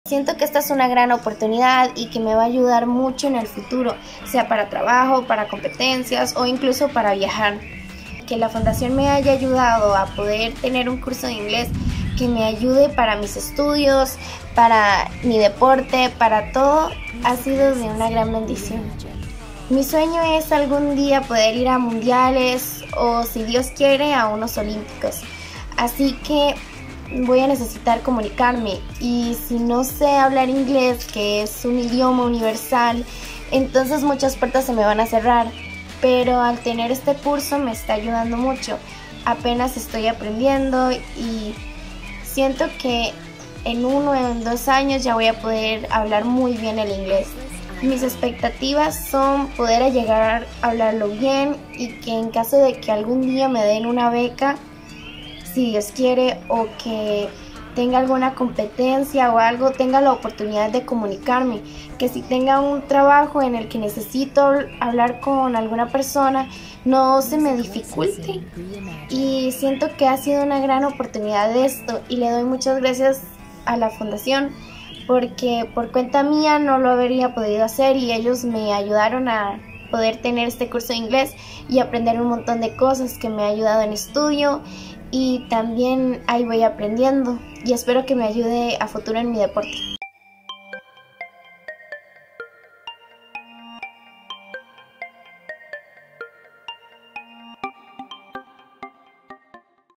ULaSalle / Abigail Salas
Gracias al convenio entre ULaSalle Costa Rica y Fundación CRC Endurance, 35 jóvenes están cursando un Técnico de Inglés que los mantiene muy motivados y esperanzados de su futuro.

Abigail Salas nos cuenta lo que ha sido para ella.